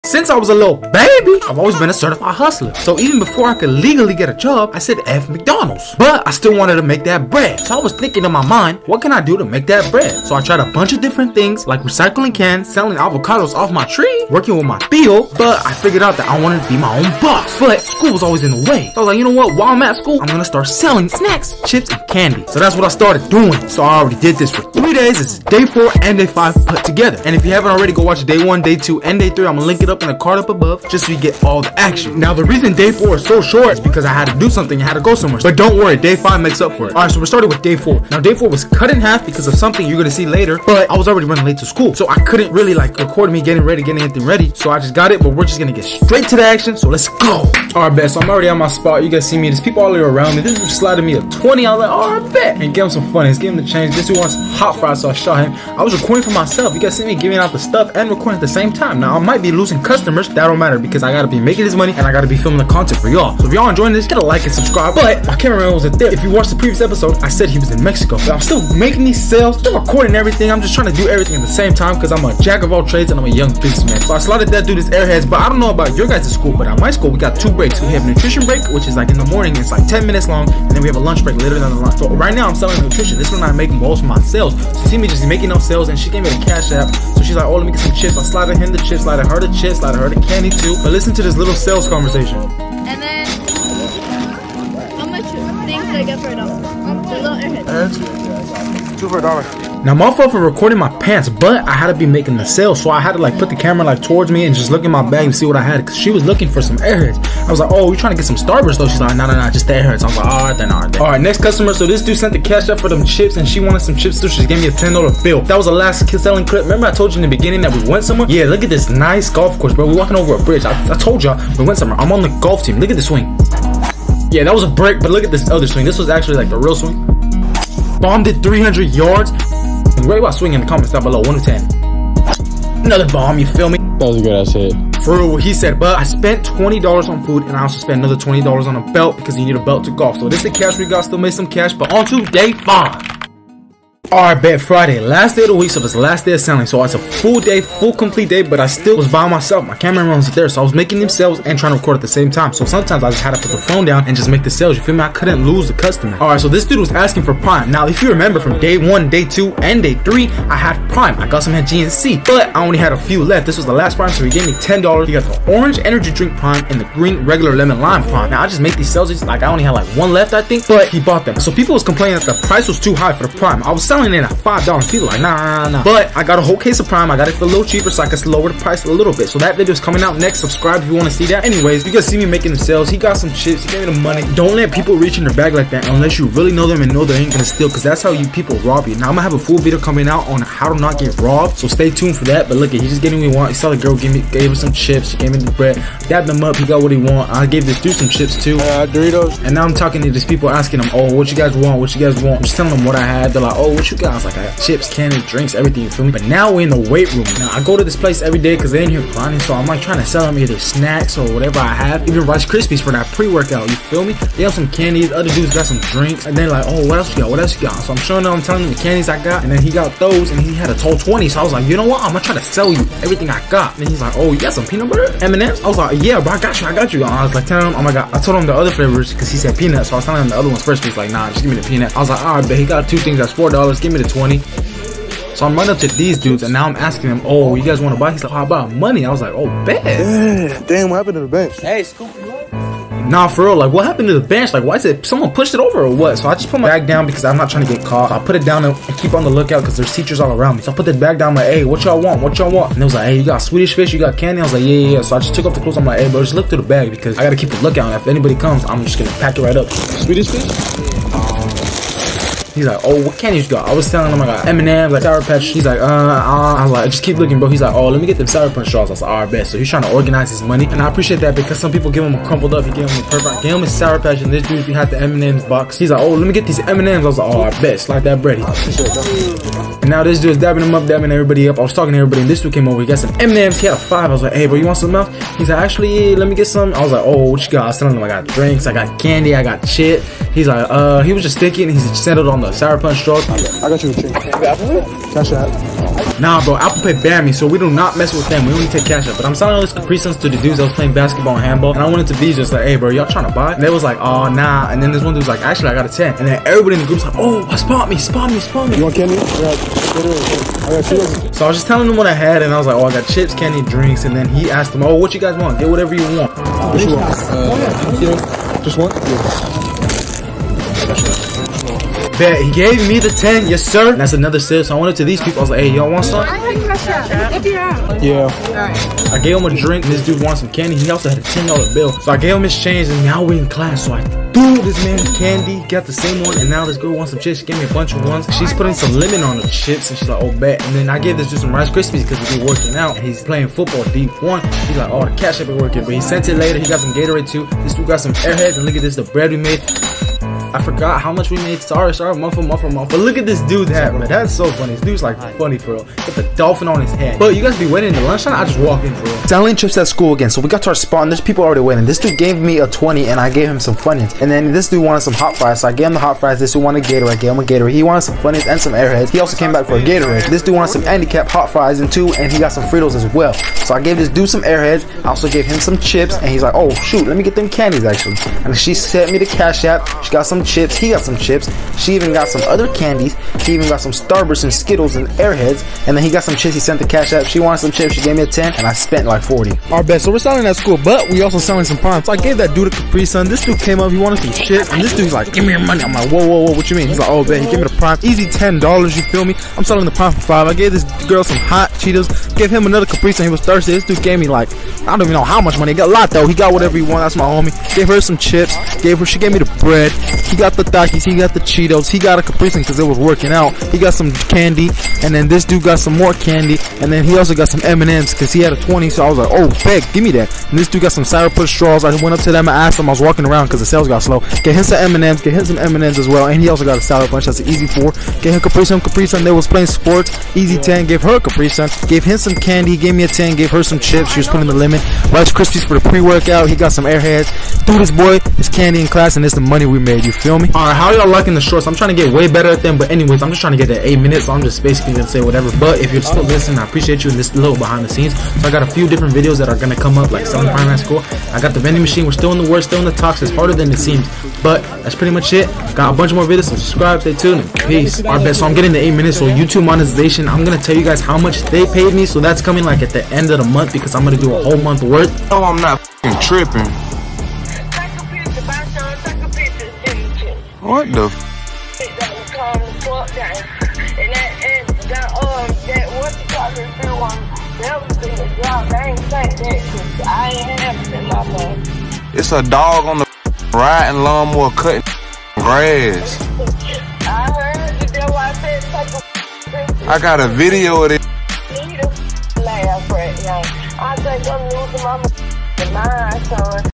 Last Day of Selling Chips at School
As a Young Entrepreneur on his Entrepreneurship Journey Selling at School is so Fun to me Because it gives me WAY MORE than what A 9-5 Can do
I am a Hustler and making Money
Learning Businss tactics and Many other things
But in this video we sold Chips, Candy, Snacks, Drinks, and Prime at School
This is Day 4 and Day 5 inthe Same Video and i Explained in the Video but Take Some Notes this is Free Game fr

If you have a question or wanna be with others that are hustling in school then join the Telegram